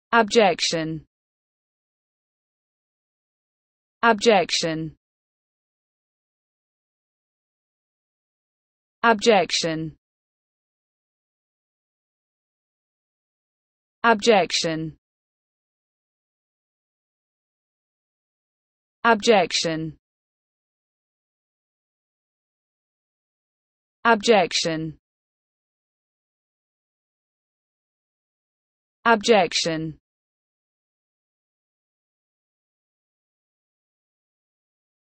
the correct pronunciation of abjection in English.
This video shows you how to pronunciation abjection in English.